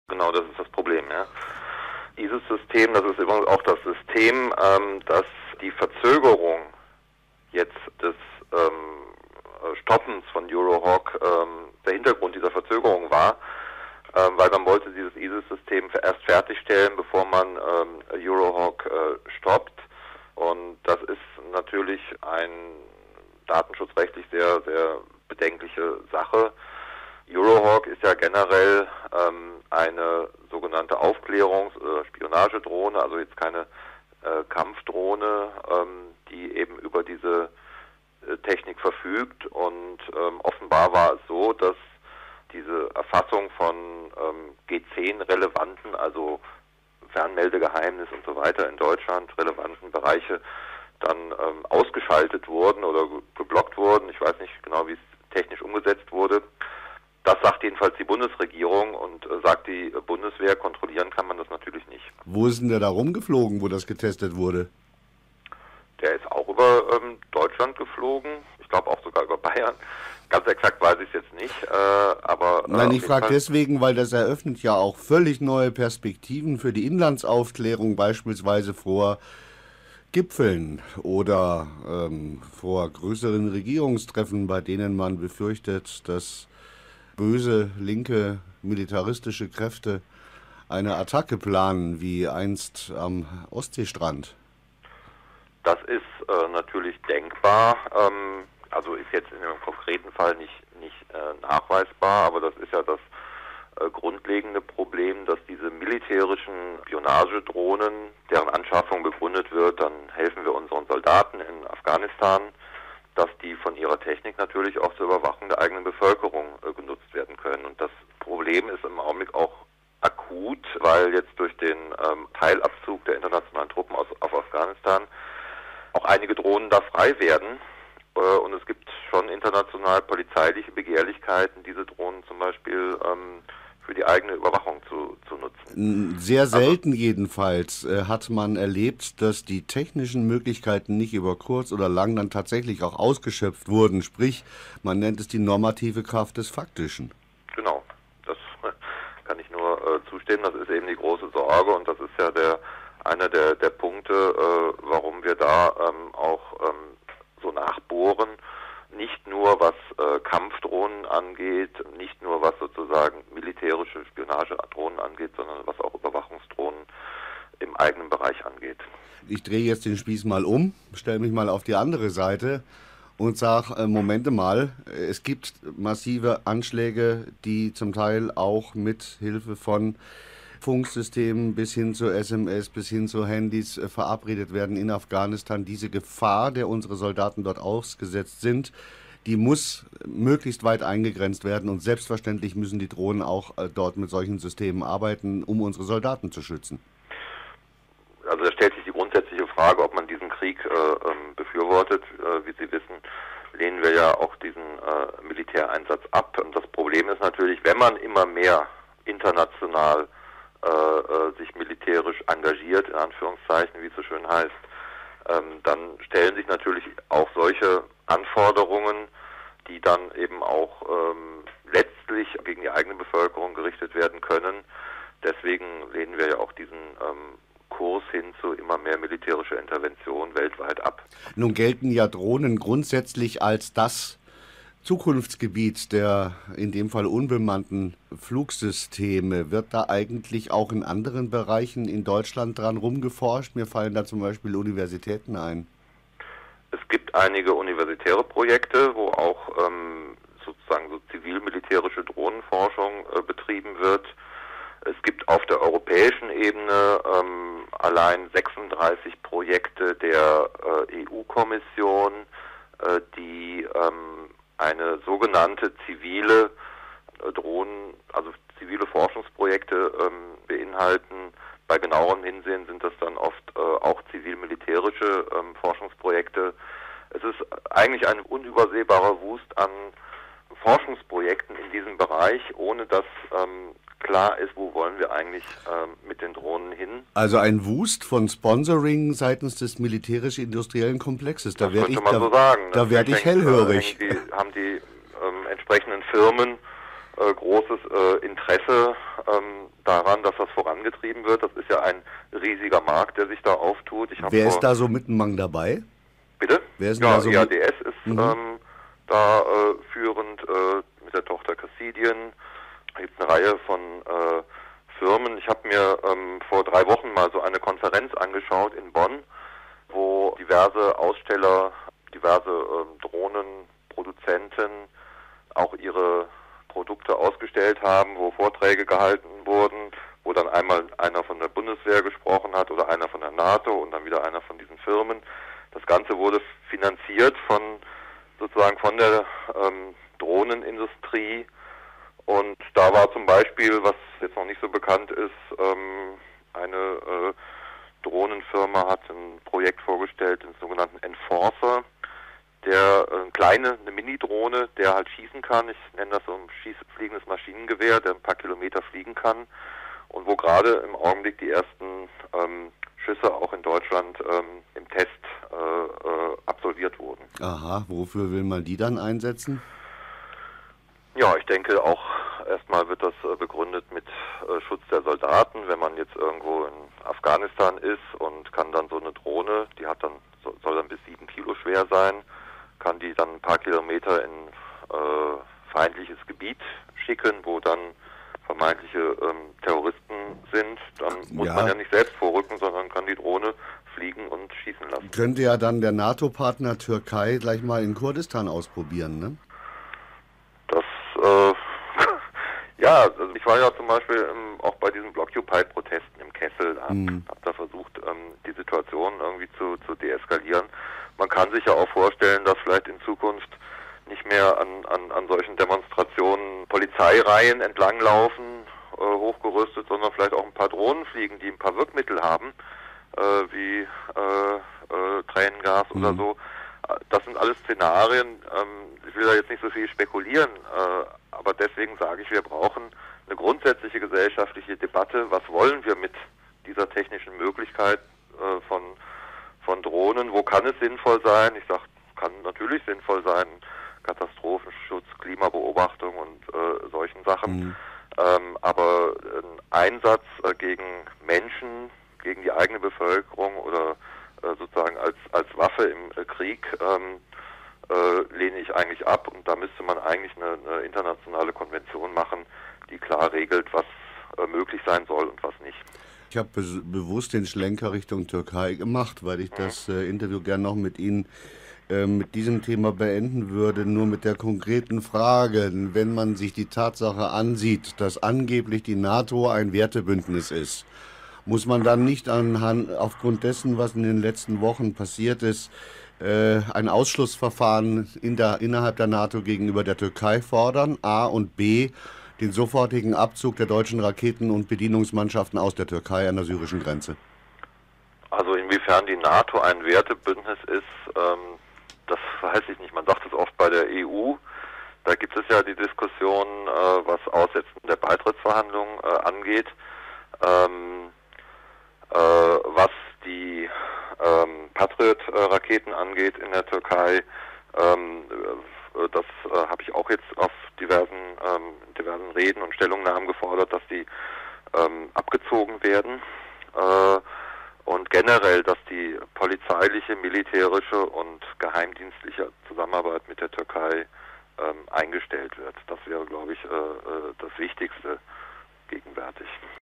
Drohnen: Andrej Hunko, Bundestagsabgeordneter - Interview - 23.6.13
Andrasch Neunert
Radio  LORA München
Laut EADS-Tochter Cassadian war die Euro-Hawk bei Testflügen über Deutschland mit dem ISIS-System zur, Zitat, „Detektion von Radarstrahlern und Kommunikationssendern" ausgerüstet. In der Antwort auf eine Anfrage nach dem Informationsfreiheitsgesetz hatte das Verteidigungsministerium zugegeben, dass dort die Erfassung von militärischen Funkfrequenzen geprobt wurde. Dabei sei, und nun aufgepasst, die Möglichkeit zur Erfassung und Auswertung von Mobilfunkverbindungen und SMS durch spezielle Vorrichtungen unterbunden gewesen.
Wir fragten den Bundestagsabgeordneten der Linksfraktion, Andrej Hunko, ob er die Tatsache, dass diese Drohnen das also längst können